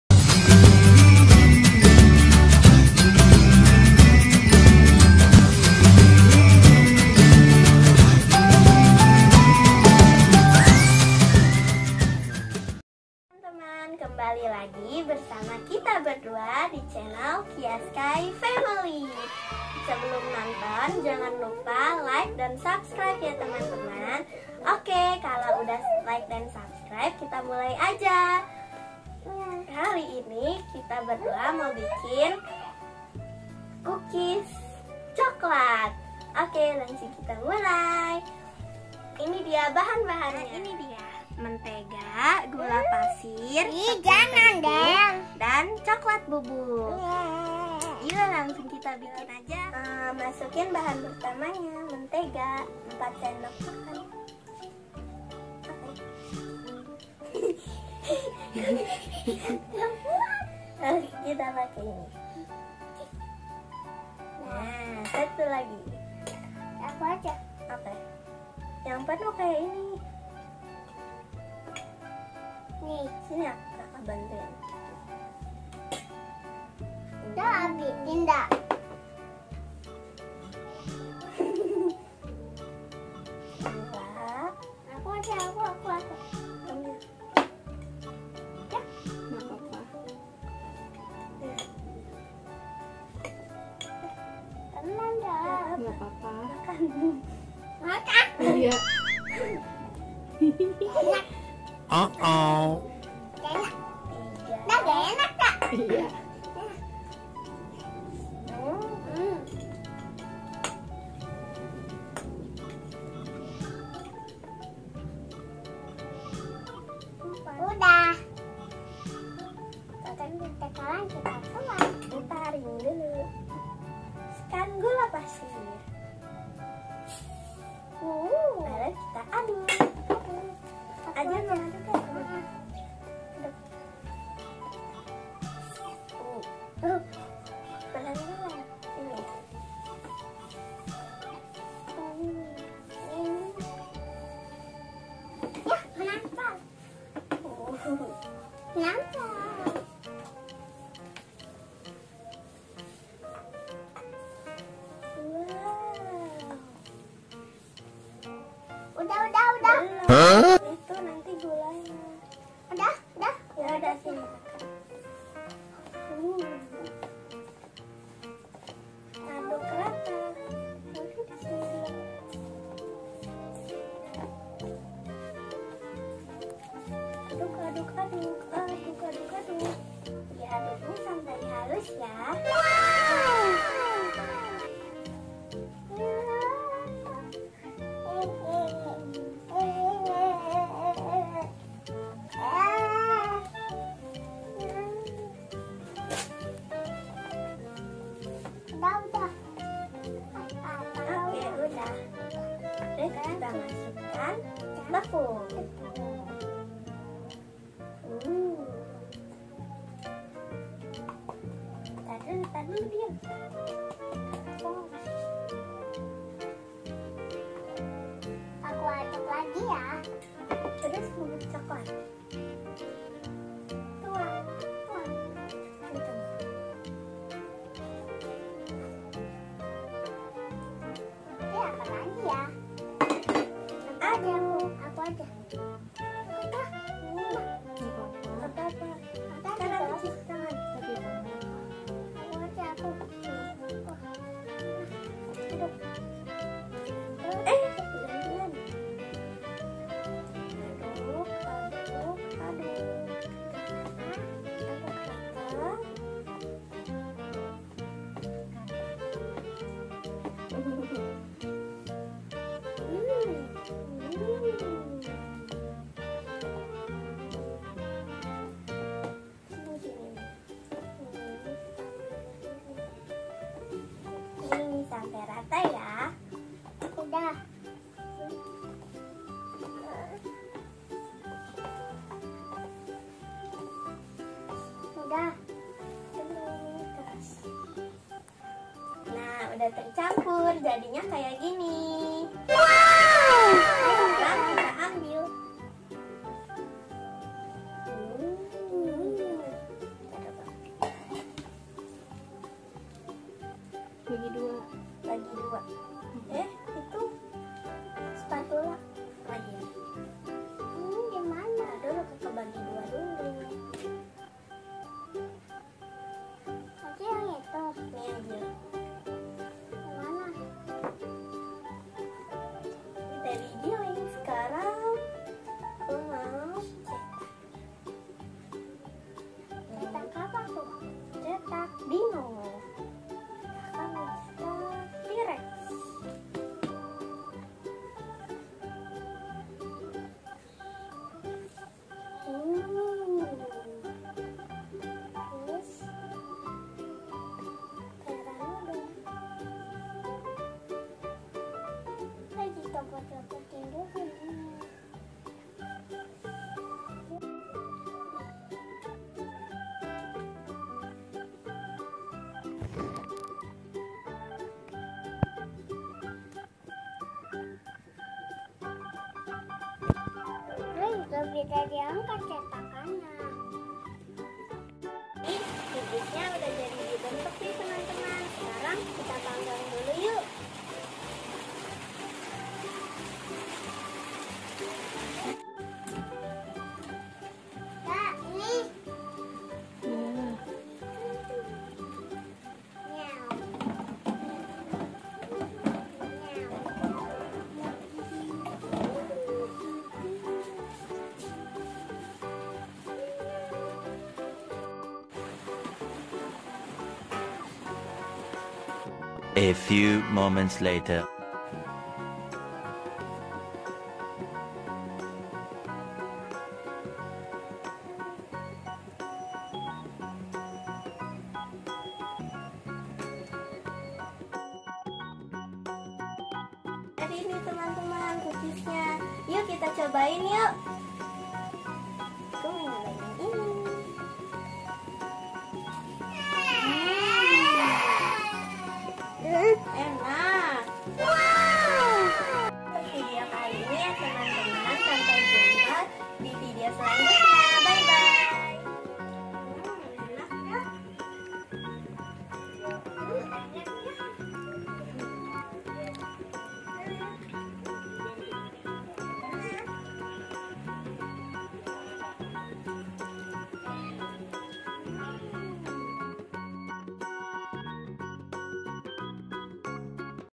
SERU!! Kegiatan Libur Sekolah Membuat Cookies Coklat Bersama
kegiatan liburan sekolah yang seru bersama qiazkay family